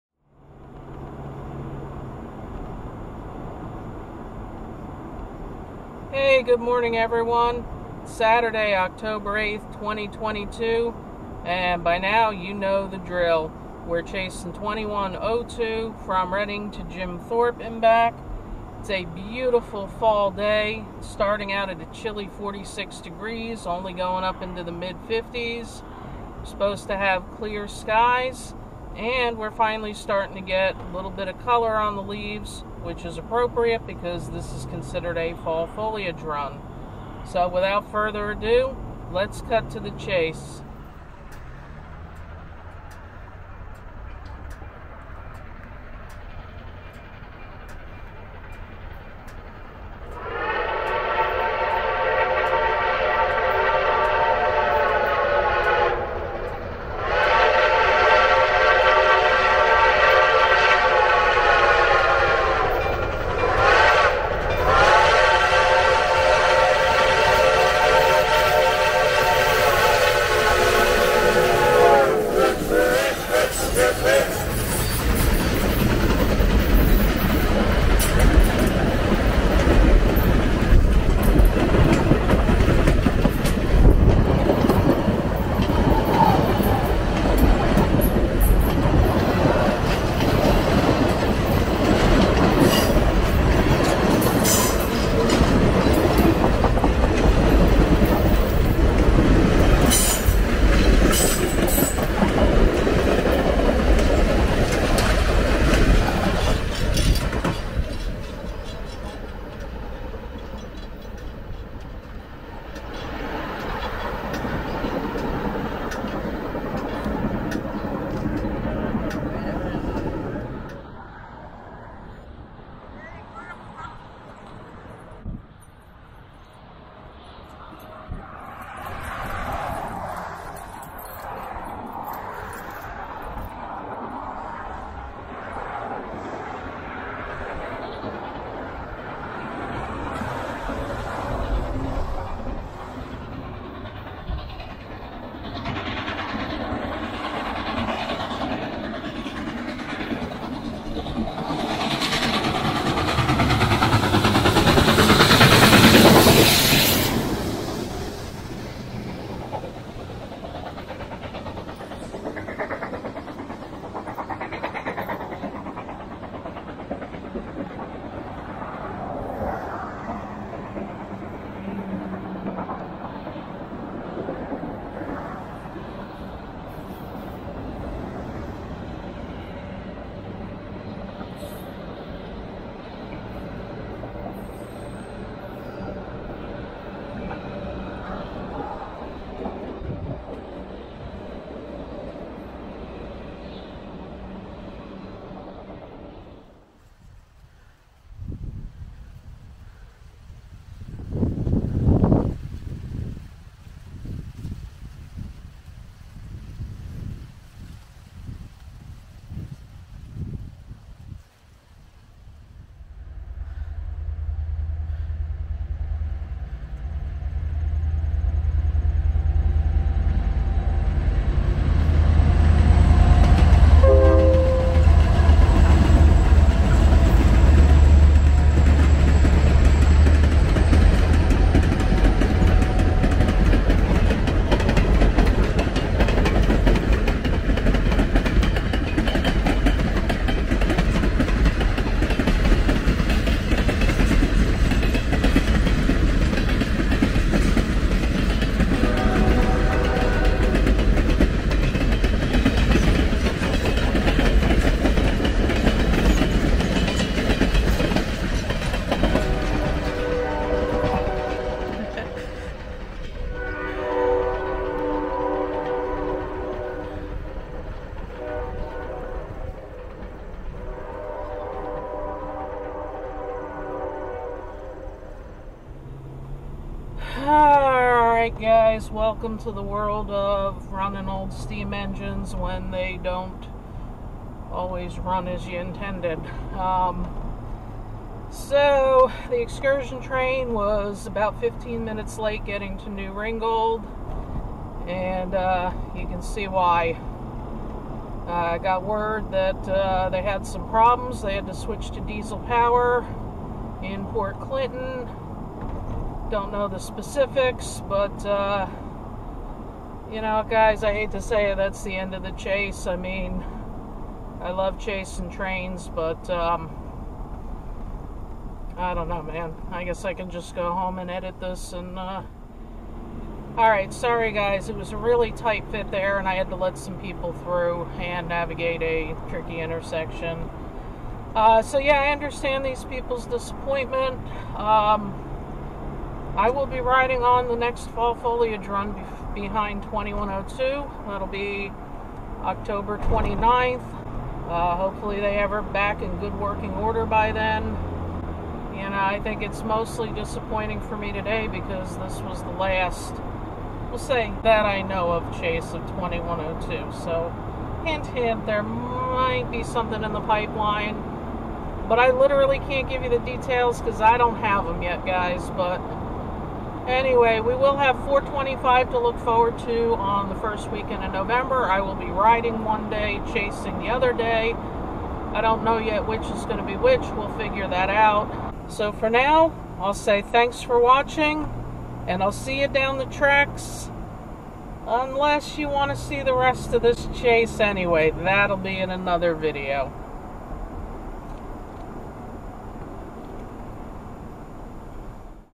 #2102's second Fall Foliage excursion - 08 October 2022
#2102's second Fall Foliage excursion of 2022 ... while it lasted!

Well ... old steam locomotives can be fickle beasts, and today it was #2102's turn to be feeling under the weather. As usual, she left Reading Outer Station at 9:00 am pulling the excursion train towards Jim Thorpe. Unfortunately, she had some sort of mechanical problem, and they switched to diesel power at Port Clinton.

For the rest of the day, the Fast Freight SD50-2's - #5018 and #5019 - pulled the excursion train. These are beautiful diesels, repainted in 2020 after being acquired from CSX. I have been wanting to railfan these for a while, but this wasn't how I wanted it to happen.

Although this was clearly disappointing to all of us - me, other railfans, the passengers, and certainly the Reading & Northern crew - these kinds of things happen with old trains. It is my understanding that Reading & Northern generous credited each passenger the difference between a regular diesel ticket and the #2102 ticket that they can use towards a later excursion.

Even though I talked about ending the chase and going home early, I soon came to my senses, recalling that there were still more places on the way home (sort of, lol) that I could catch the train. I put these segments in a separate video that will be fully diesel powered.

#2102 will pull one more Fall Foliage trains on October 29. The remaining excursions will be pulled by the diesel F units except for November 5 and November 6 which will be pulled by the #425 Light Pacific 4-6-2 steam locomotive.

Use these timestamps to jump ahead to the location where I caught the train:
0:00 intro
0:41 Bellemans Church Road, Dauberville
2:01 Route 61 over Old US 22, Hamburg
3:17 East Railroad Avenue, New Ringgold ... uh oh!
4:16 outro

Chasing trains and building models is hard work! You can help me refuel and support my adventures by buying me a coffee!